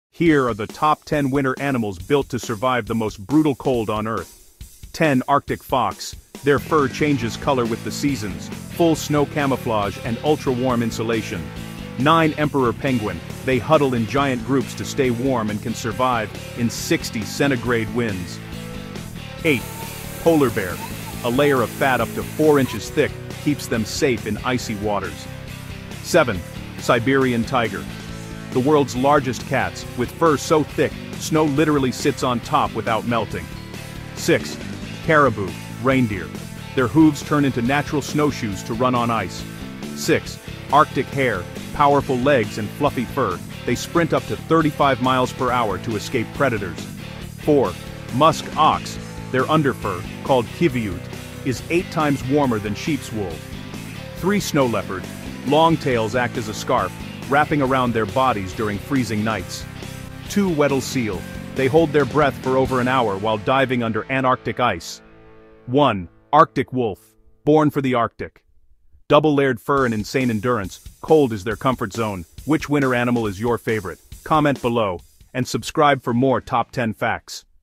Top 10 Winter Animals Built for Extreme Cold
These animals are built for the coldest places on Earth! From Arctic wolves to emperor penguins, discover the Top 10 animals perfectly adapted to freezing temperatures. 🌍❄️
👇 Comment your favorite cold-weather creature!